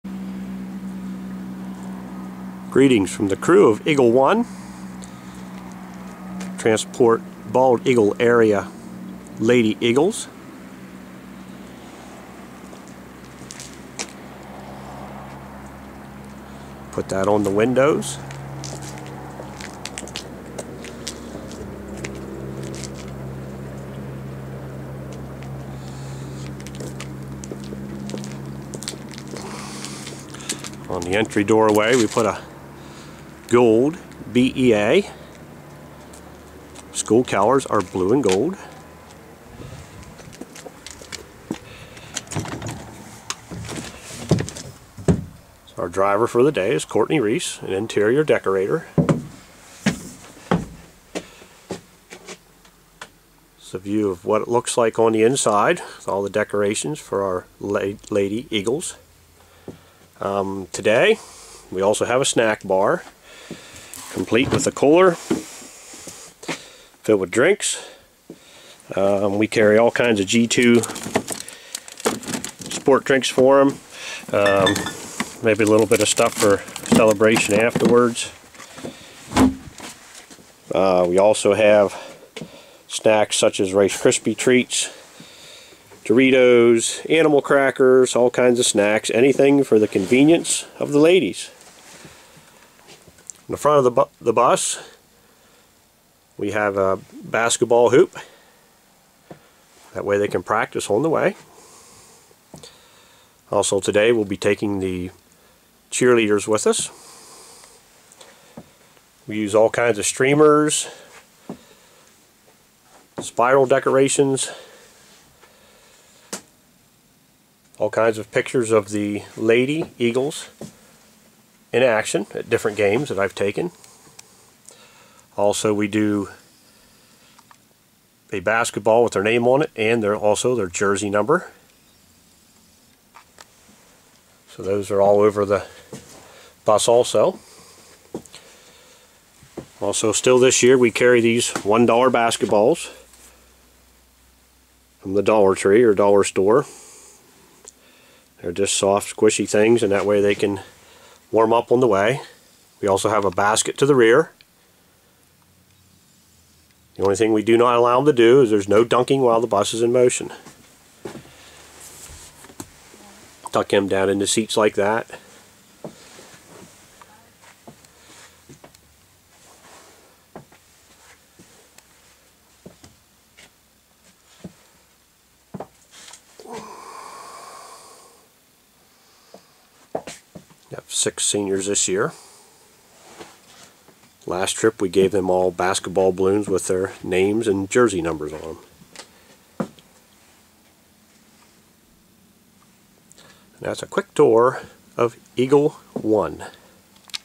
Eagle One Rides Again
Bald Eagle Area Lady Eagles travel to games in style. Streamers, pictures and decorations inside the bus create a positive game attitude. Snacks and a variety of drinks are in the front seat. Baskets placed in the front and back provide extra warm up time. Small plush basketballs are used to provide shooting practice. However, slam dunking is not allowed. #1 Transportation for our #1 Ladies!!! Suggestions for next years bus include1) 3D video projection screen, 2)Blue and Gold carpet,3)serving Chill-n-Grill's 50 inch Pizza,4) Wi-Fi, and 5) two strong guys to carry and load the water jug onto the bus and  who would be able to help sing the Alma Mater upon arrival at our destination high school.  WHO'S GONNA WIN??  WE'RE GONNA WIN!!!  GO!  FIGHT!  WIN!